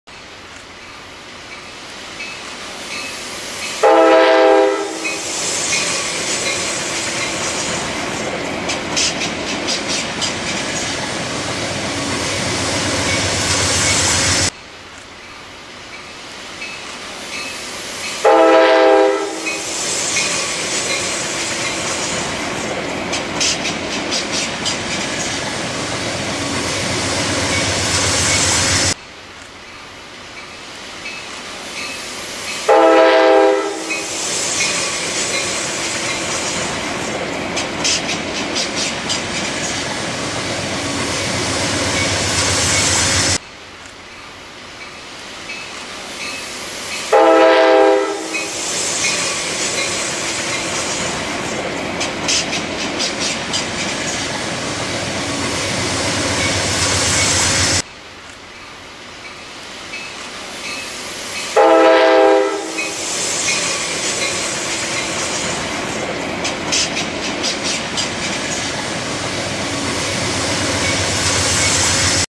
Passenger train almost hit dancing animals by the river
The train almost hit the animal on the tracks, but thank God everything went well